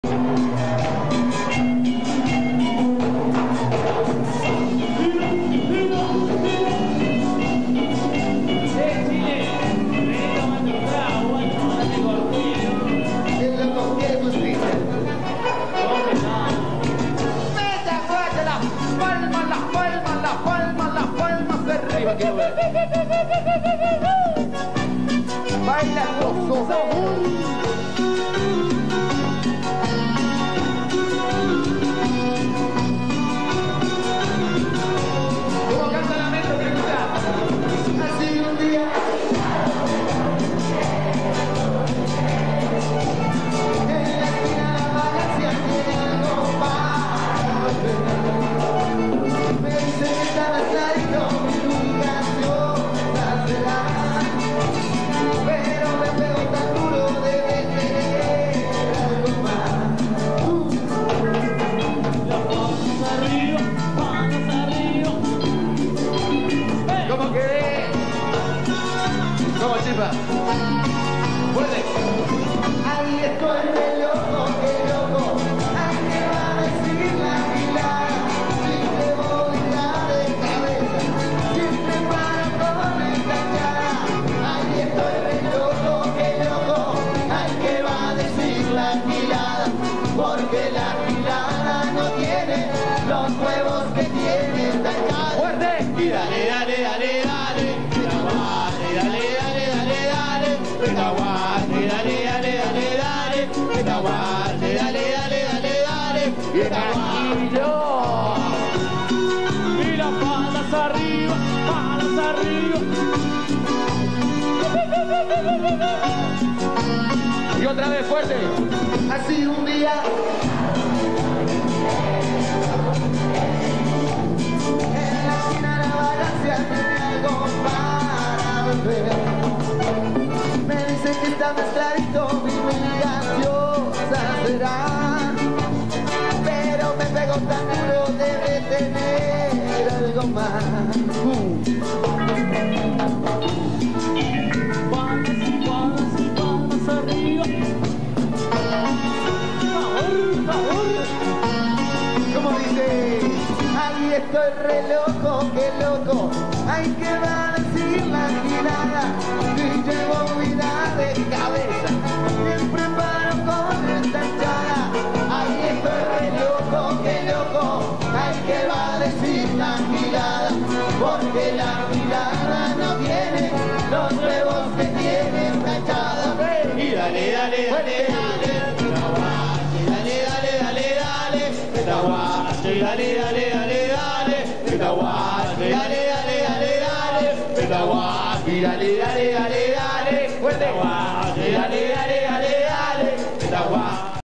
METAGUACHA- VIDEOCLIP EN VIVO- LA METRO 2001. INEDITO WIROCRISPIANI.
VIDEOS INEDITOS DEL RECUERDO DE LA MOVIDA TROPICAL ▶OTRA JOYA Y VAN...😉✔ METAGUACHA- VIDEOCLIP EN VIVO!!! - LA METRO. SUBIDO POR WIRO Y SU BASE🎵🎶